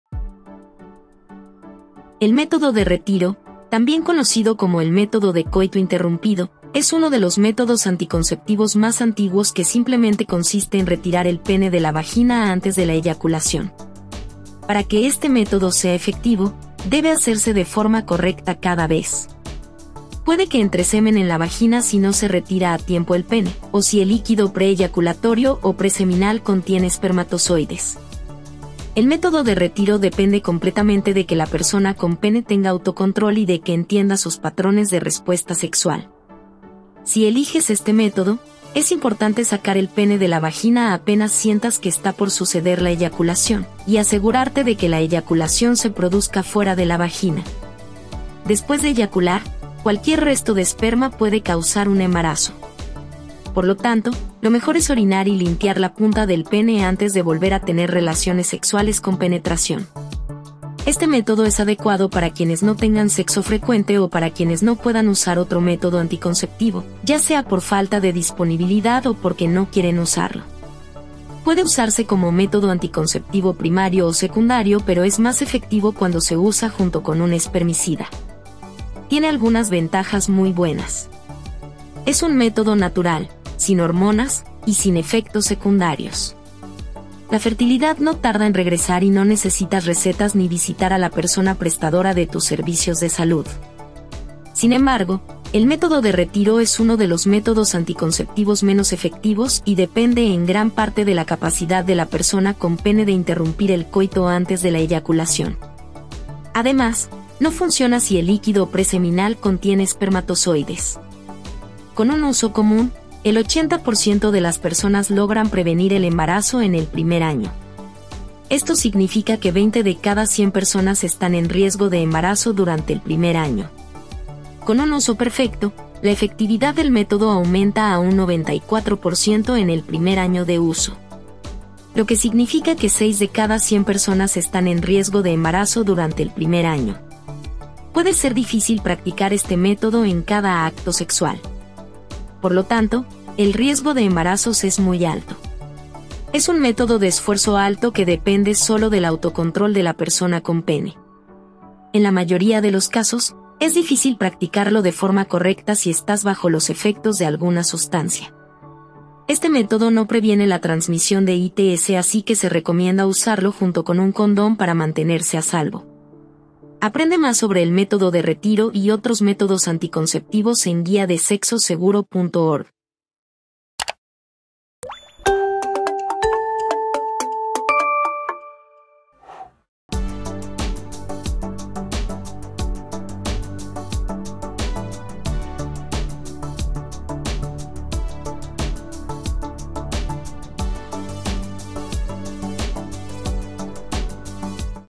Método de retiro (coito interrumpido)
El método de retiro, o coito interrumpido, es un anticonceptivo antiguo y simple donde la persona con pene retira el pene de la vagina antes de eyacular. Para que sea efectivo, debe hacerse correctamente cada vez. Descubre más sobre cómo funciona y sus consideraciones en nuestro video.